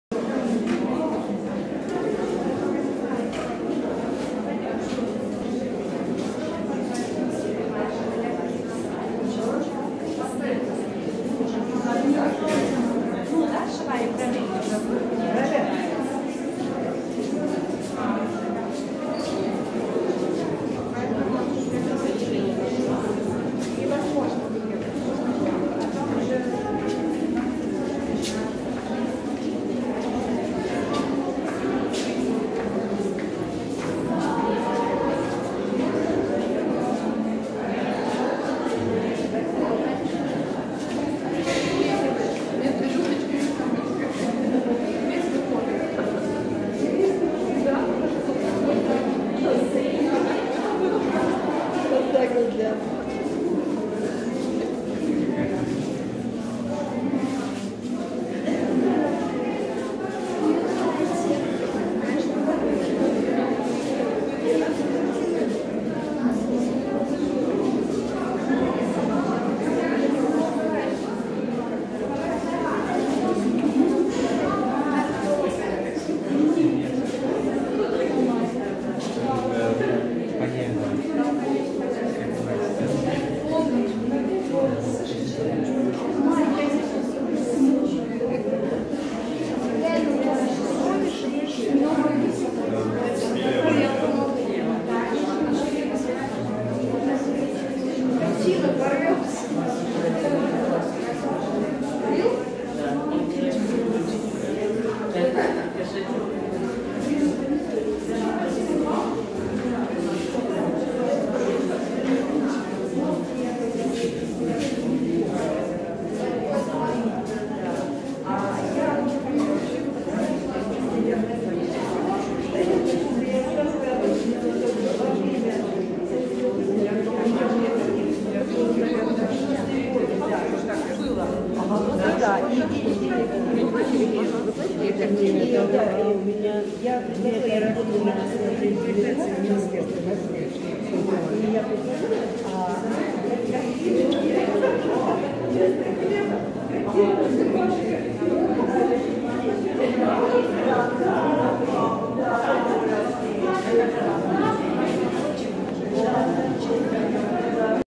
Обзор (кратко и быстро) картин Татьяны Васильевой "Цвет любви", зарисовка ч.1, 15.07.2022
Персональная выставка картин Заслуженного художника РФ Татьяны Васильевой "Цвет любви", Обзор, ч.1, Национальная галерея Республики Коми, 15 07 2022